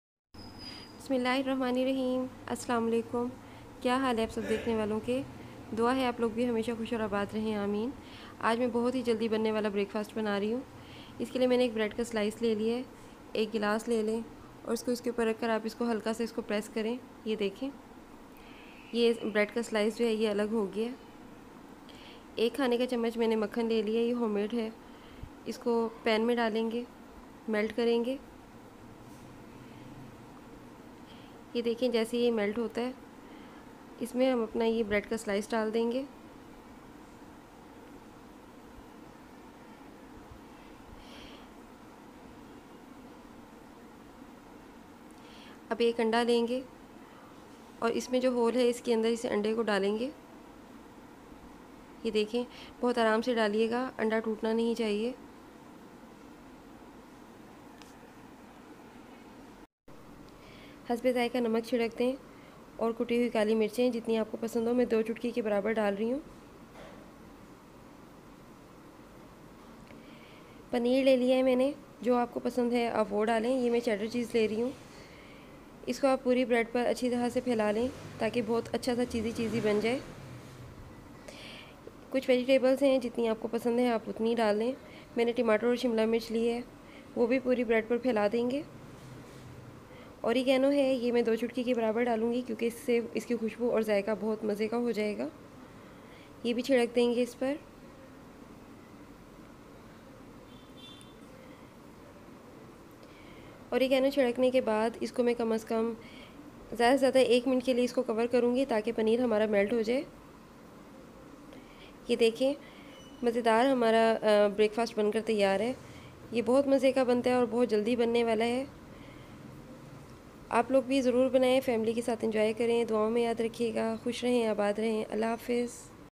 Quick break fast made by Fozia Nasir
Ingredients
Bread slice 1
Egg 1 Salt
Black pepper
Origano 2 pinch
Chadder cheese
Salt
Shimla mirch choped
Tomato choped